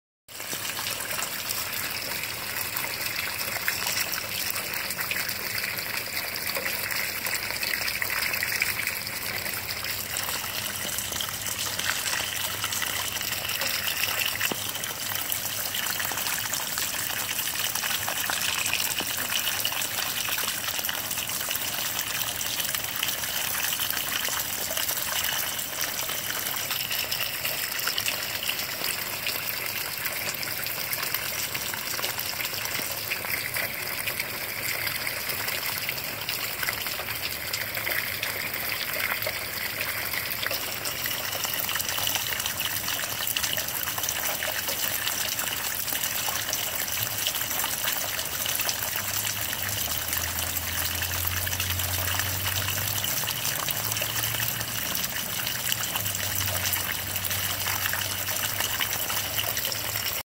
Rainwater Downspout
A light rain sure added up when ypur roof channels it to the eaves and then the downspout. {9-6-2017}